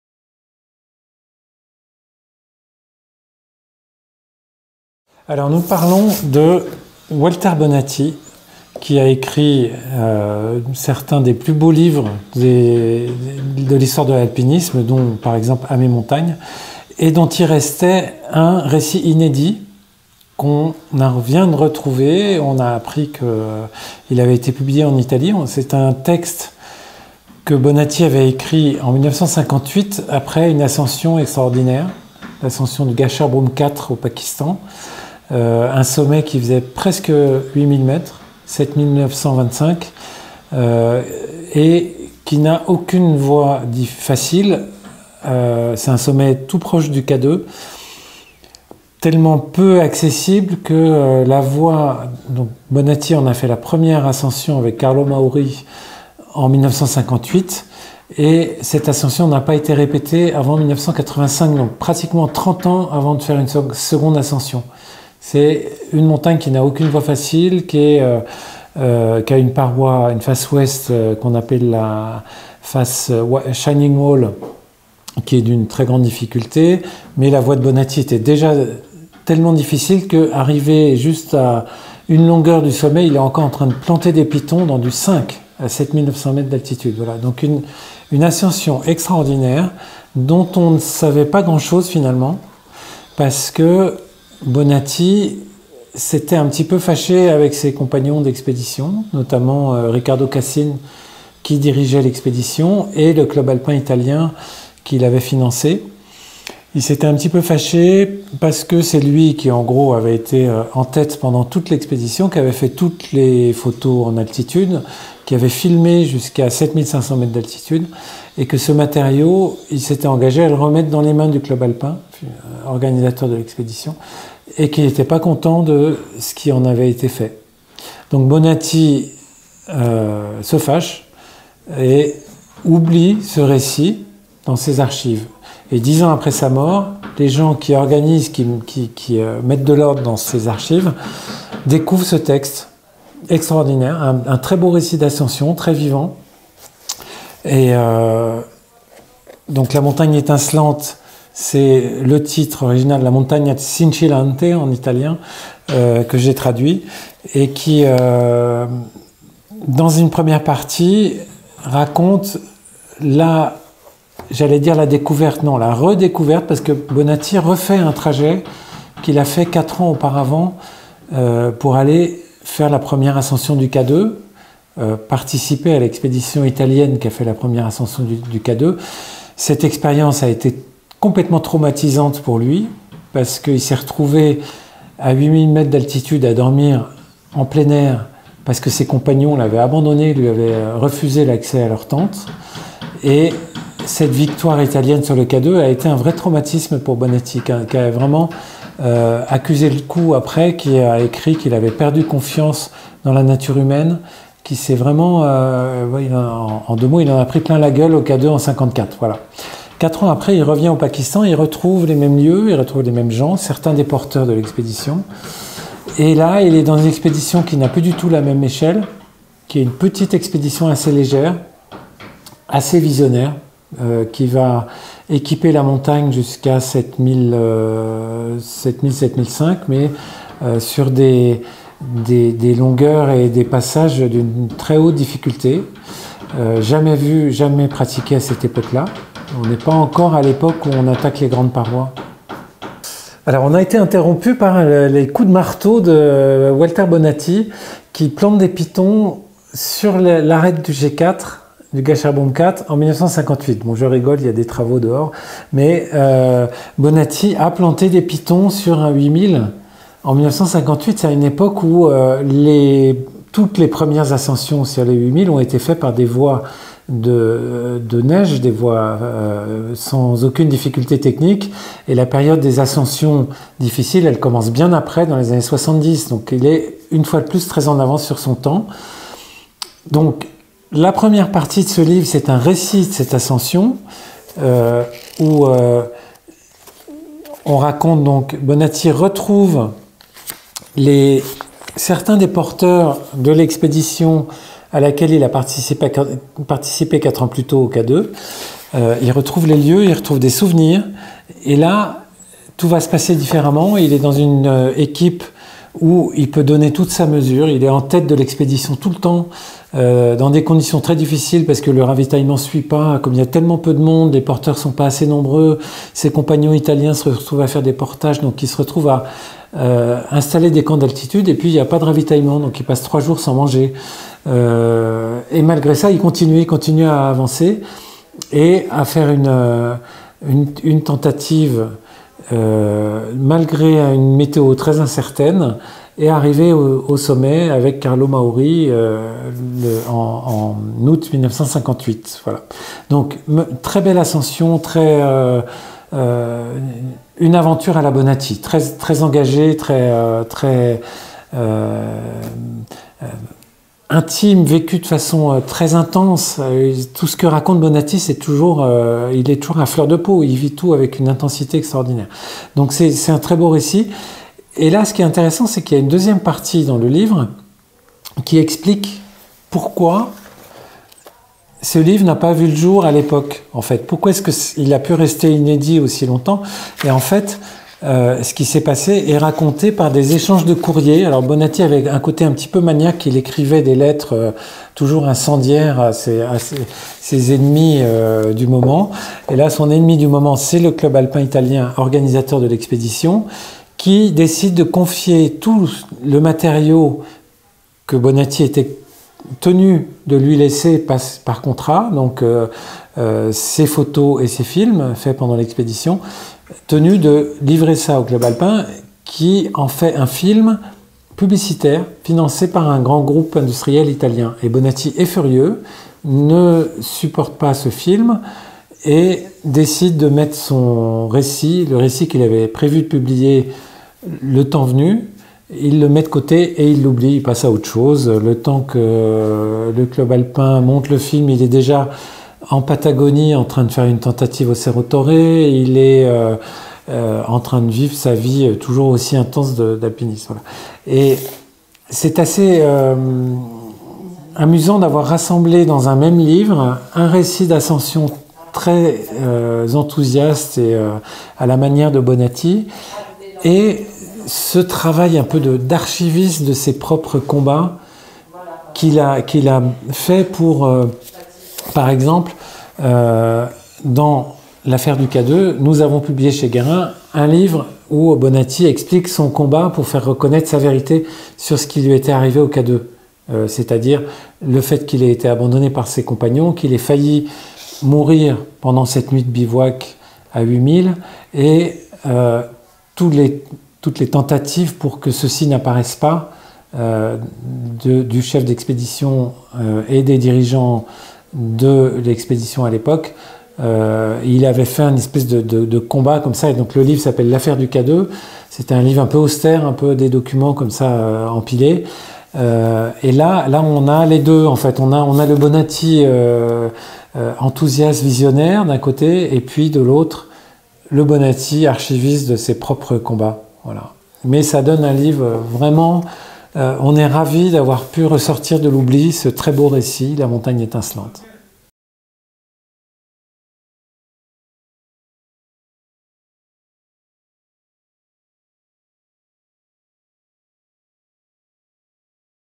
La montagne étincelante Walter Bonatti Editions Paulsen Guérin Chamonix littérature alpinisme
Mai 2023, livre, montagne, alpinisme, himalaya... La montagne étincelante, un livre inédit de Walter Bonatti aux Editions Paulsen Guérin Chamonix... L'ascension du Gasherbrum IV, 7925 mètres... Il est situé à la frontière de la Chine et du Pakistan dans la chaîne du Karakoram... Quelques mots de Charlie Buffet le traducteur du livre... Littérature, culture...
- Un inédit de l'immense alpiniste italien Walter Bonatti.
En août 1958, Walter Bonatti signe avec Carlo Mauri une ascension légendaire au Pakistan. Le Gasherbrum IV, 7 925 mètres !  Quelques mètres lui manquent pour se placer dans la fameuse liste de quatorze 8 000, mais les alpinistes savent que cette montagne-là n’a aucune voie facile. Sa difficulté sidérante la place au-dessus de la plupart des autres. Pour Bonatti, l’ascension est un exorcisme, quatre ans après l’expérience traumatisante vécue sur le K2, tout proche, où ses compagnons l’avaient abandonné une nuit en pleine tempête.
Si cette ascension magistrale, que personne ne parviendra à répéter pendant presque trente ans, était passée presque inaperçue, c’est en partie parce que Bonatti avait été empêché de publier son récit, comme les contrats d’édition le stipulaient souvent à l’époque.
Dix ans après la mort de ce géant de l’alpinisme, son récit inédit a été retrouvé dans les archives du musée de la Montagne, à Turin. L’auteur de À mes montagnes s’y exprime d’une plume intense, où les sentiments et l’action s’épaulent mutuellement pour créer un récit captivant. Qui n’a pas pris une ride.